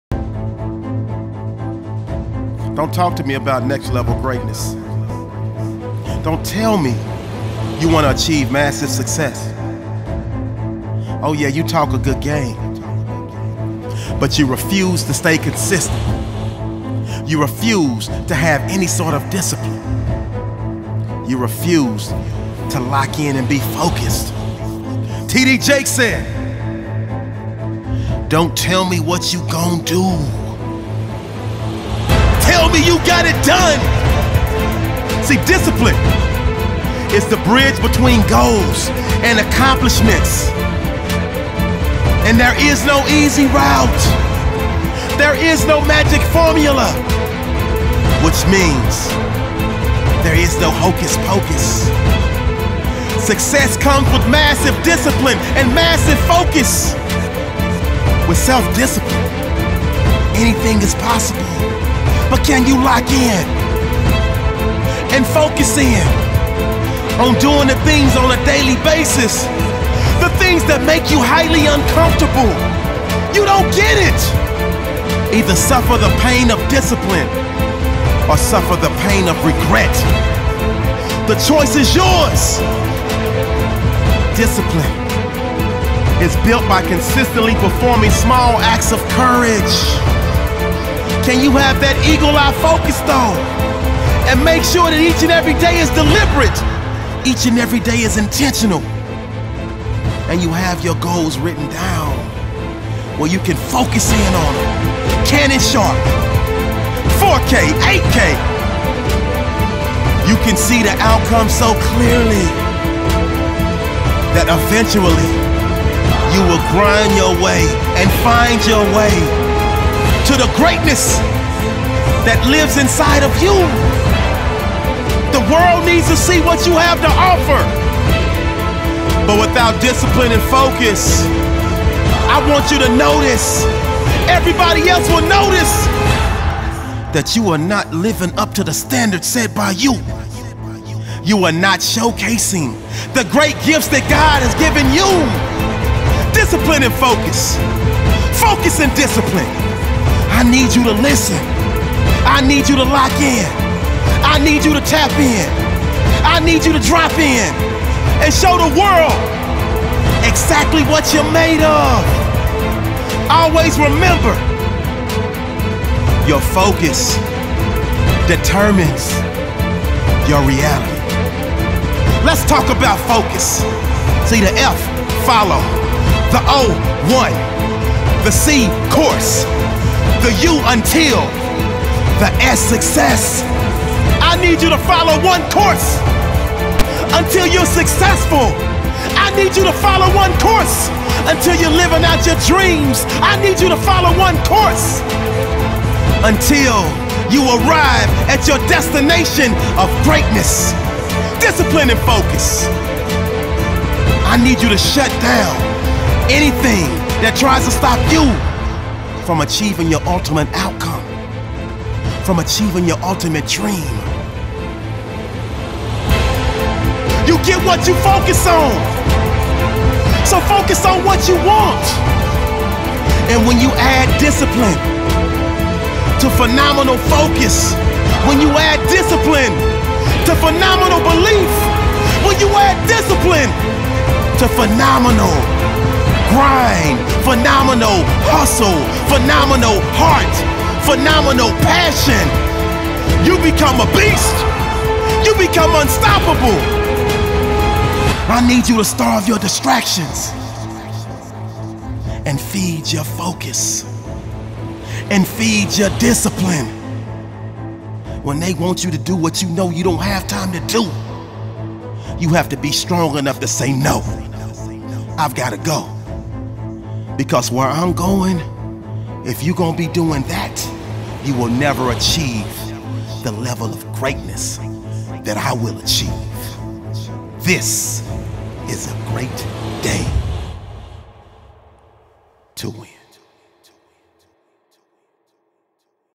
Discipline And Focus | Gym Workout Inspiration | Freddy Fri Motivation
In this video, I want to teach you how to stay disciplined and focused in all aspects of your life!  My name is Freddy Fri and  I welcome you to Freddy Fri Motivation!

This episode is designed to push you to be disciplined and focused in all aspects of your life.